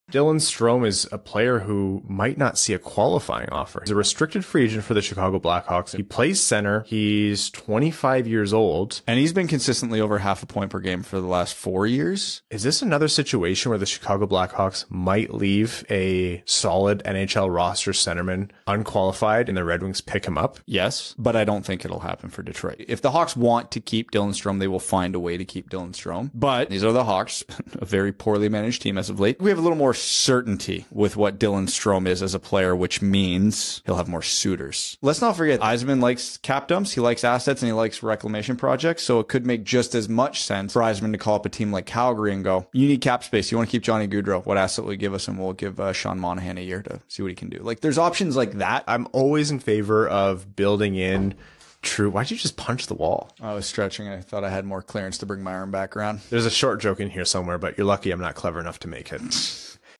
Could Yzerman bring Dylan Strome to the Detroit Red Wings?
Could Dylan Strome be Yzerman's solution for a new middle-6 center for the Detroit Red Wings? If Chicago even leaves him unqualified, would he come to Detroit? Or would Yzerman be better suited taking Monahan off of Calgary's hands? Let us know what you think! ⤵️